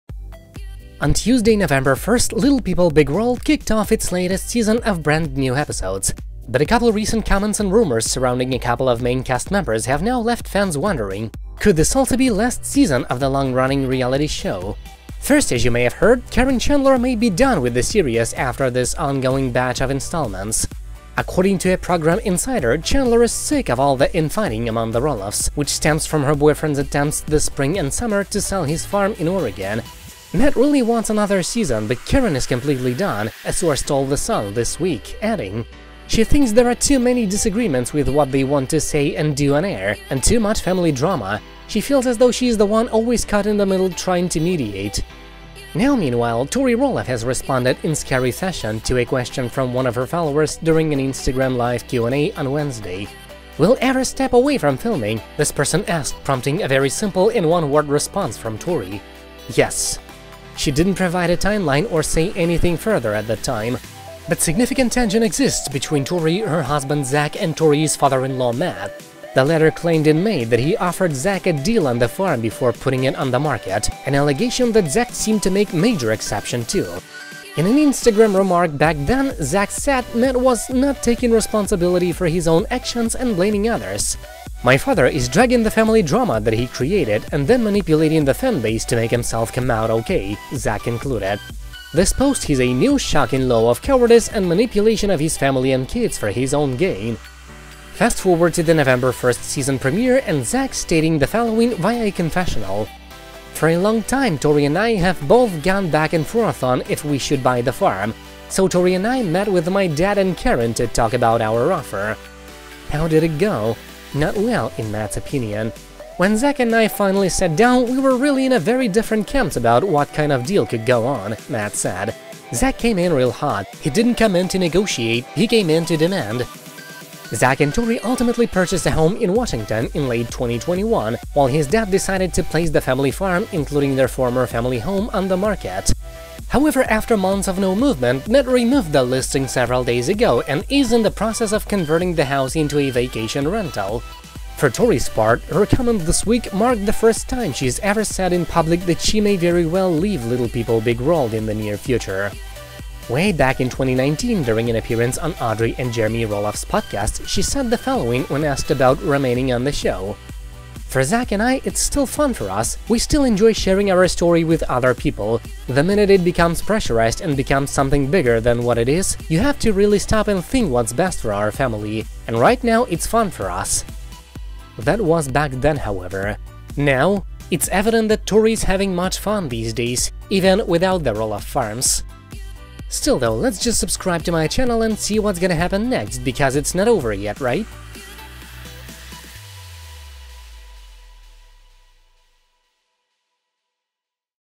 We Leave Little People, Big World... Soon! - Tori Roloff
On Tuesday, November 1, Little People, Big World kicked off its latest season of brand new episodes.

But a couple recent comments and rumors surrounding a couple of main cast members have now left fans wondering:

Could this also be last season of the long-running reality show?!?